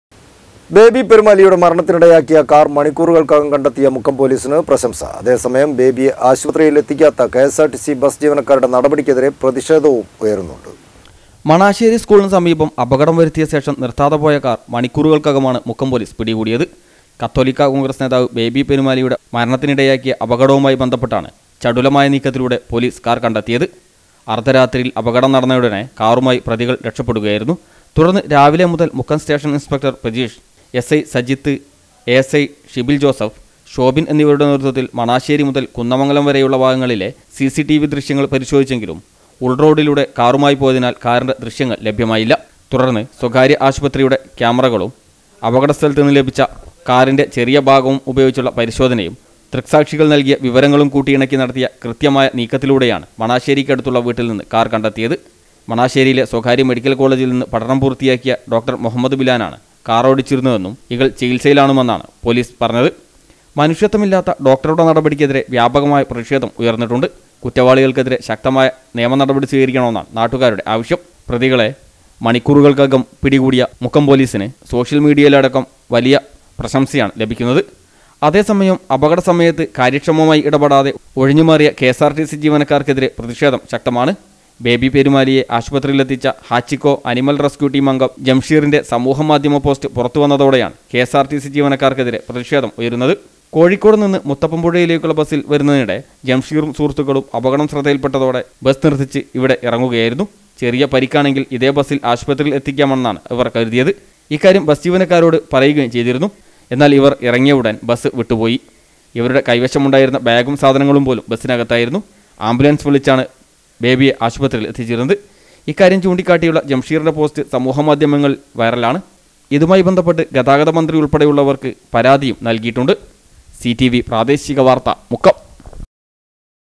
കാർ മണിക്കൂറുകൾക്കകം കണ്ടത്തിയ മുക്കം പോലീസിന് പ്രശംസ
ബേബി പെരുമാലിയുടെ മരണത്തിനിടയാക്കിയ കാർ മണിക്കൂറുകൾക്കകം കണ്ടത്തിയ മുക്കം പോലീസിന് പ്രശംസ.അതേസമയം ബേബിയെ ആശുപത്രിയിലെത്തിക്കാത്ത കെ.എസ്.ആർ.ടി.സി
ബസ് ജീവനക്കാരുടെ നടപടിക്കെതിരെ പ്രതിഷേധവും ഉയരുന്നുണ്ട്.